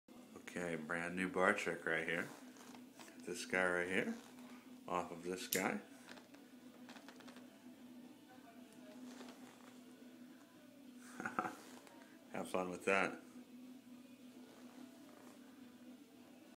Beer bottle top - Bar Trick
Spinning a beer bottle on it's cap.  Fun & creative bar trick!!